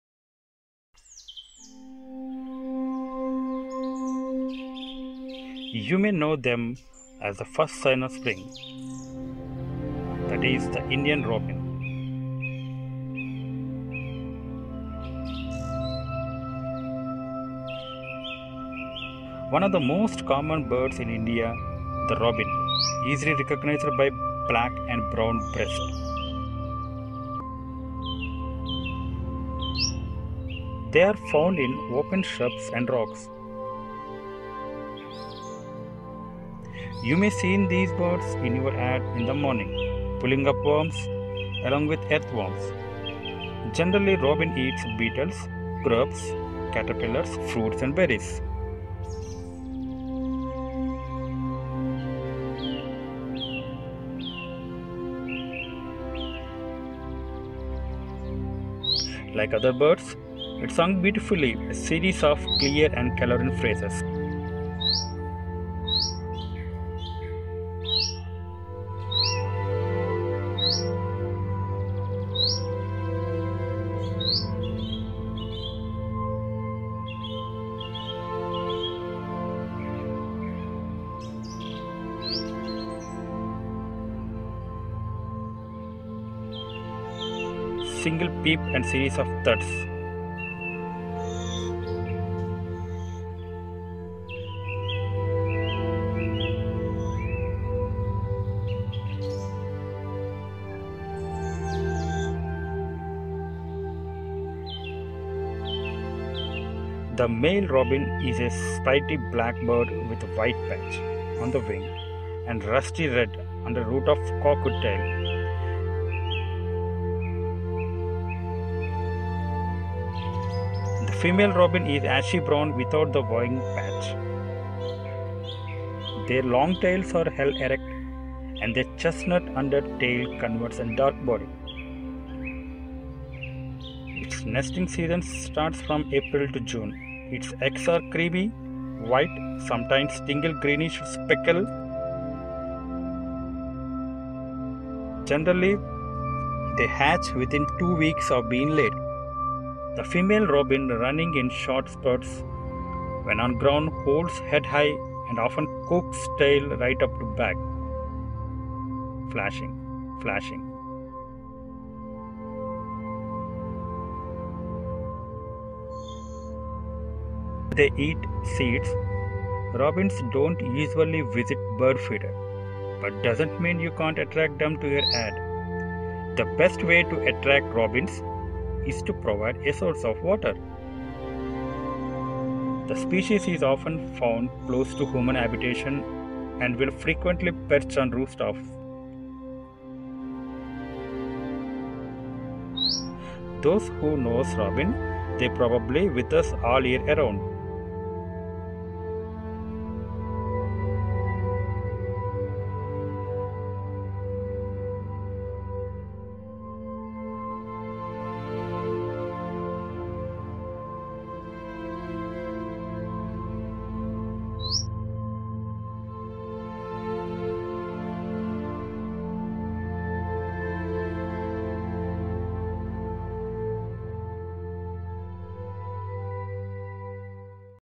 Indian Robin Bird - Documentary
The Indian robin documentary inspired  from National Geographc channel , it was shot,edited, narration, color graded by chandra sekhar during lock down period in the backyard.

Robin is a sprightly black bird with white patch on the wing and rusty red under root of cocked tail. Its hen is ashy brown without the wing-patch. It can be seen in pairs in dry open lightly wooded country. It feeds on insects and their eggs, spiders etc. It's nesting season ids from April to June. Its nest is pad of grass, rootlets and rubbish lined with feathers or hair, often with bits of snake slough as adornment placed under stone, in hole in earth bank or tree stem or within a derelict tin can or earthen pot. Its eggs are creamy white sometimes tinged greenish speckled and blotched with reddy brown.